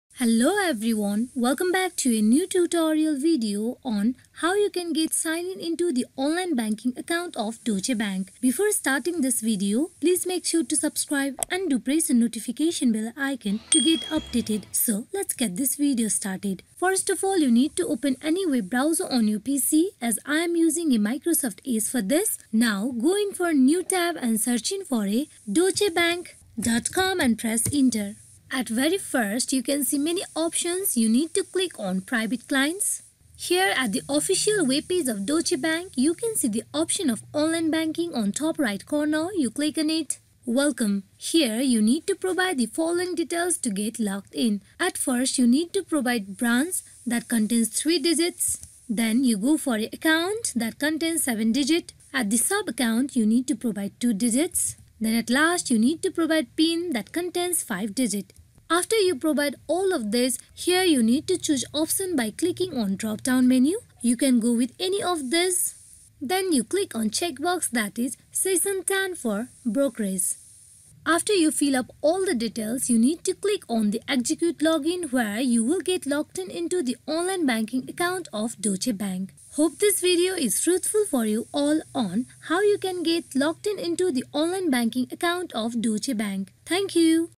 How To Login Deutsche Bank Online Banking 2021 | Deutsche Bank Online Account Sign In Help | db.com
"How To Login Deutsche Bank Online Banking 2021 | Deutsche Bank Online Account Sign In Help | db.com" - A Video Guide by How To Tutorials.

This tutorial video guides you on how you can get sign in to Banking account of Deutsche Bank.
1. Open any browser and search in for deutschebank.com and press ‘Enter’.
2. Click on Private Clients.
3. At the webpage, click on Online Banking on top right corner.
4. Provide details that is:
Branch (3 digit)
Account (7 digit)
Sub- account (2 digit)
PIN (5 digit)
of your banking account. Click ‘ Execute Log In’.
You will get logged in to your banking account of Deutsche Bank.